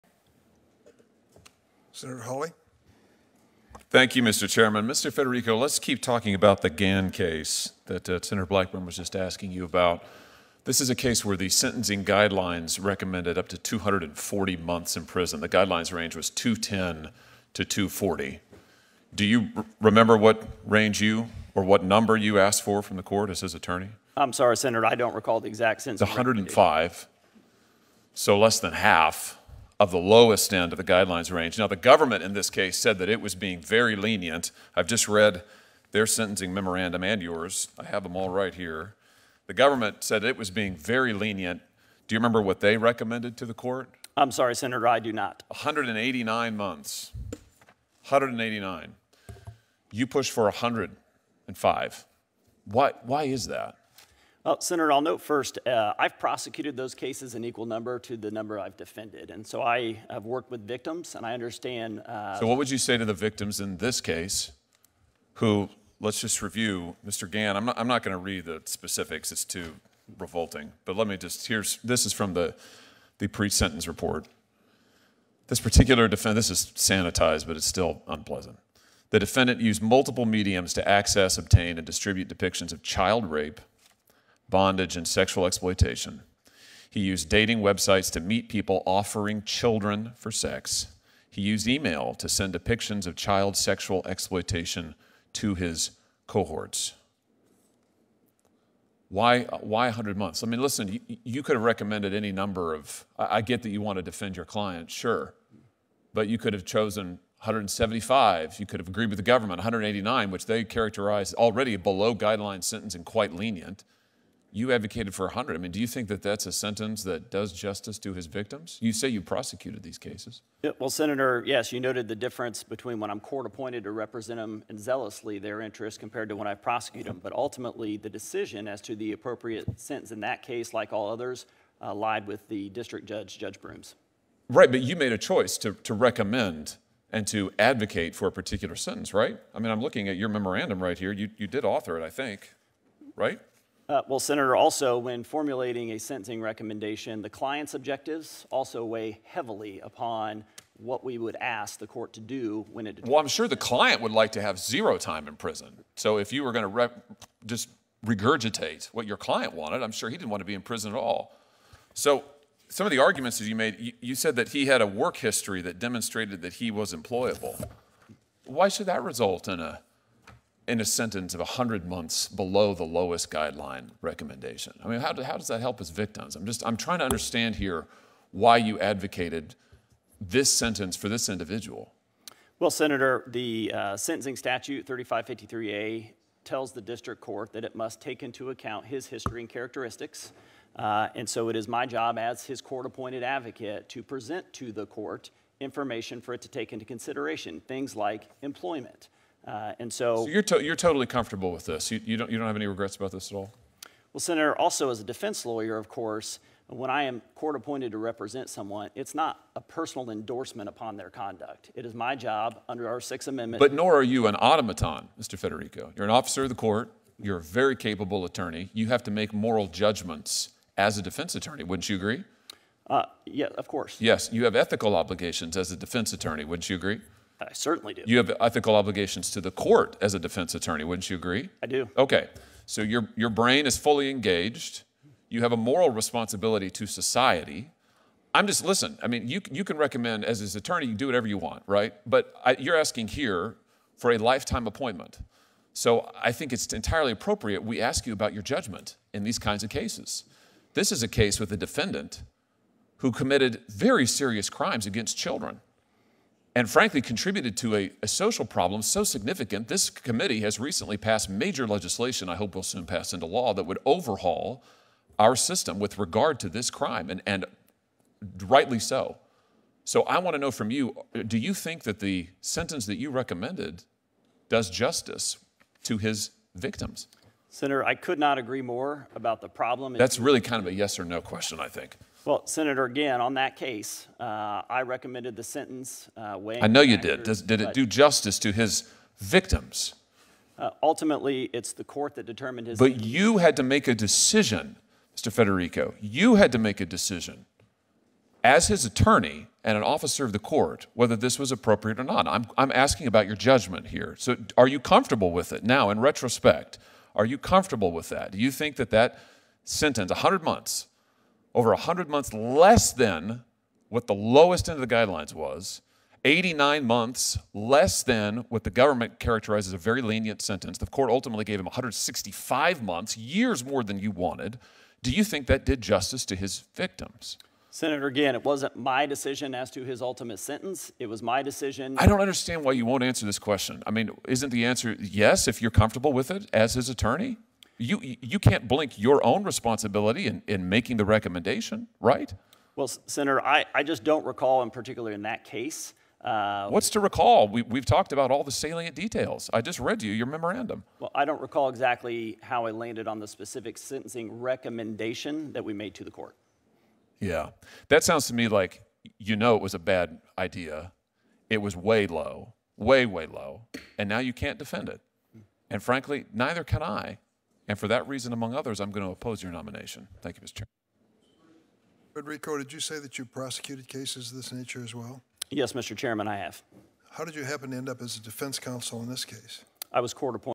Hawley Grills Nom About Trying To Get Low Sentence For Man Guilty Of 'Revolting' Crimes Against Kids
At today's Senate Judiciary Committee hearing, Sen. John Kennedy (R-LA) questioned Richard E. N. Federico, nominee to be United States Circuit Judge for the Tenth Circuit, about his beliefs, about his helping a client guilty of crimes against children get a comparatively low sentence.